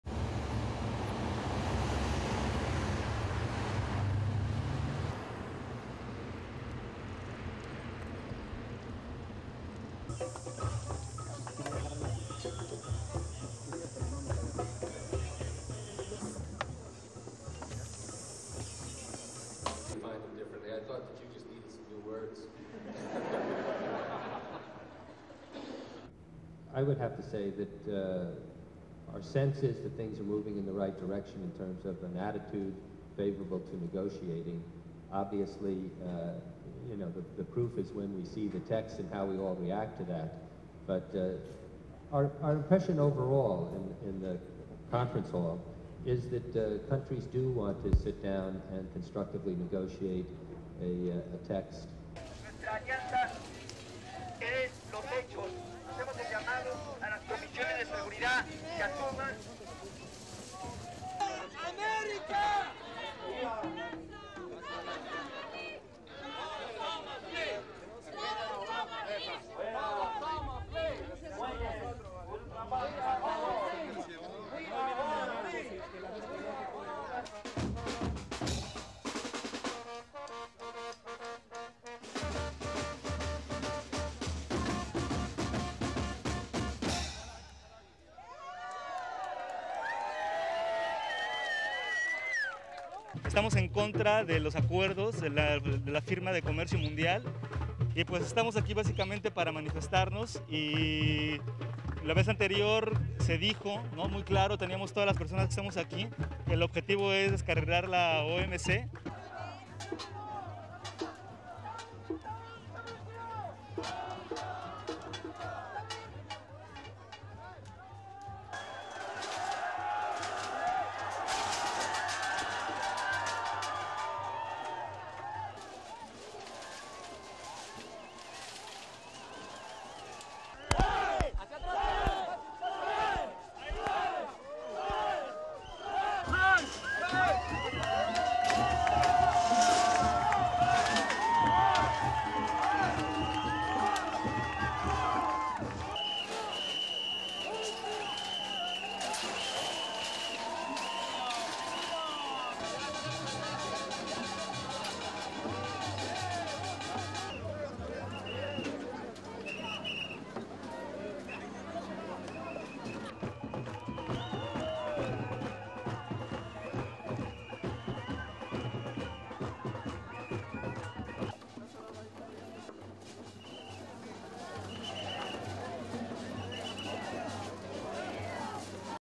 Anti-globalisation march amid reports of progress in WTO talks
1. Various of convention centre
2. Police vehicles and personnel at barricade
3. Policemen watching barricade
4. Media conference with U.S. Deputy Trade Representative, Peter Allgeier
5. SOUNDBITE: (English) Peter Allgeier, U.S. Deputy Trade Representative:
"I will have to say that our sense is that things are moving in the right direction in terms of an attitude favourable to negotiating. Obviously the proof is when we see the text and how we all react to that. But our impression overall, in the conference hall, is that countries do want to sit down and constructively negotiate a text."
6. Protesters marching
7. Protestor spraying anti-WTO graffiti on wall
8. Demonstrators playing music
9. SOUNDBITE: (Spanish) Alan Medina, Mexican Protestor:
"We are against the accords made by the WTO, we are here to demonstrate this. Before, it was said, not very clearly by people opposed to the WTO, that we are here against this."
10. Protesters against barricades
11. Protestor cutting into barricade
12. Protesters pulling barricade down
13. Police in front of barricade being pulled down
14. Protesters pulling on ropes to bring barricades down
15. Demonstrators at barricade with police
16. Protesters pulling barricades aside with police looking on
17. Anti-WTO banner
18. Anti-WTO effigy
19. Protesters cheering
20. Protestor wearing sky mask

STORYLINE:

Several thousand protesters marched on a meeting of the World Trade Organization on Saturday, tearing down a barricade erected to keep them out of the site of the trade talks.

Demonstrators tore down the first line of defence erected by officials in Cancun to keep out mass protests from the convention site where talks are being held, but police refrained from physically confronting the crowd.

The World Trade Organization issued a draft resolution on Saturday that would force member countries to cut subsidies to farmers, but it didn't go as far as many developing nations have demanded and many said it was unacceptable.

All 146 members of the global trade organization were reviewing the proposal, and were scheduled to discuss and amend it through Sunday.

The final document - put together by the meeting's chairman, Mexican Foreign Secretary Luis Ernesto Derbez - could have a major impact on the world's farm trade.

Developing countries, led by Brazil, India and China, had asked for the elimination of all subsidies for farm goods being exported, and major cuts in subsidies for domestic farm products.

But the proposal appeared to stop short of what they asked.

The negotiations have sparked widespread protests around the world in the past, and this round appears to be part of that pattern.

Protesters have promised to attend each day of the current round of negotiations to express their rejection of what they call "neo-liberal" concepts of global trade.

U.S. Deputy Trade Representative Peter Allgeier told a news conference on Saturday that progress is being made and "there is appears to be a willingness among countries" to strike a deal.